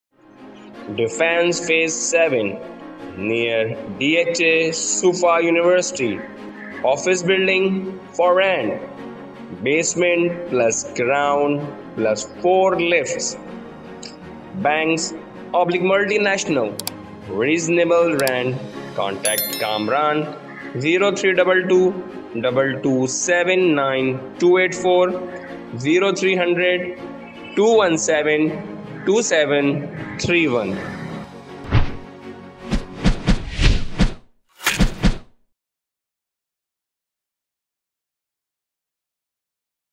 DEFENCE PHASE 7, NEAR DHA SUFFA UNIVERSITY OFFICE BUILDING FOR RENT | ZAMEEN TV | 170822
DEFENCE PHASE 7, NEAR DHA SUFFA UNIVERSITY OFFICE BUILDING FOR RENT BASEMENT+ GROUND +4 LIFT , BANKS / MULTINATIONAL REASONABLE RENT